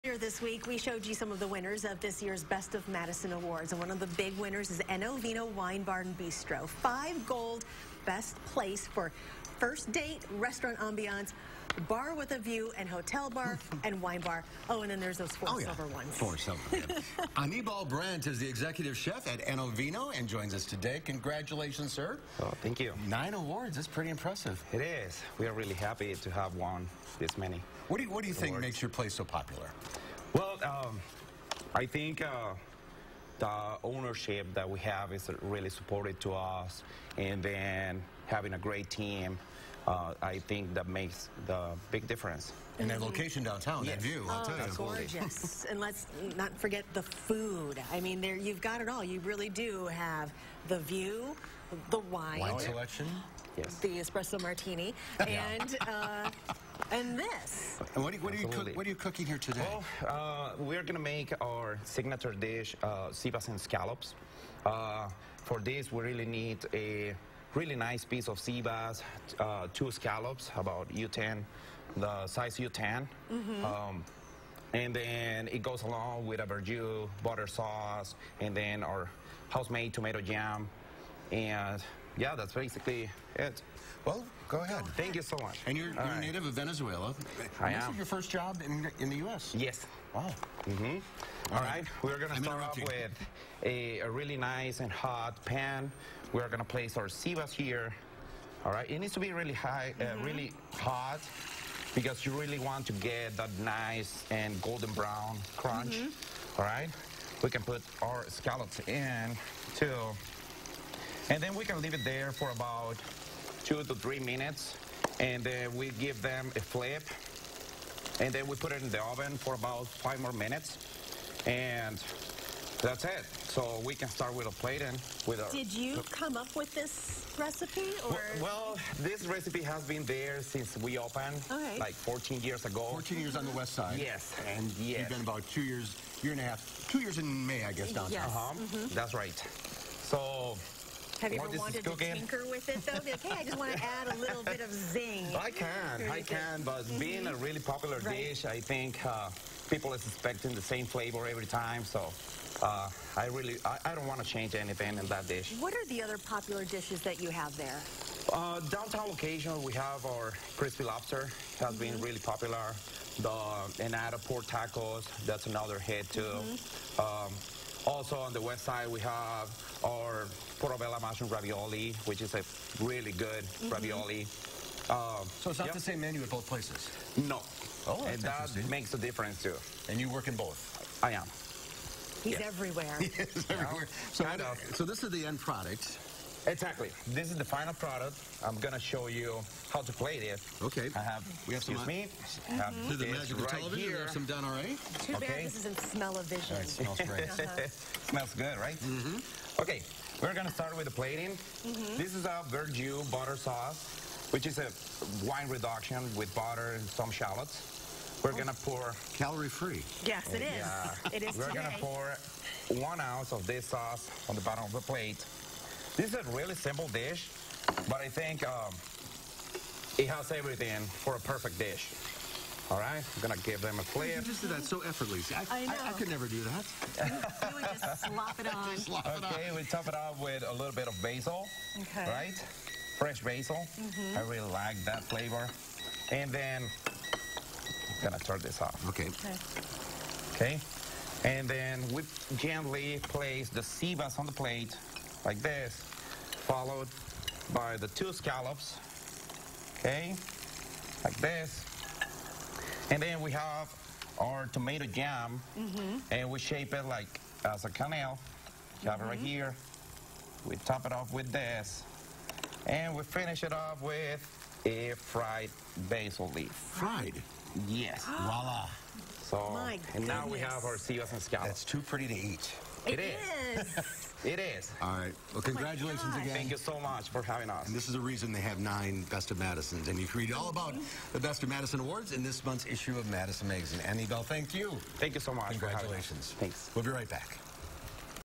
Best of Madison winner Eno Vino shares recipe for sea bass, scallops
Executive Chef Anibal Brandt from Eno Vino shares how to make sea bass and scallops.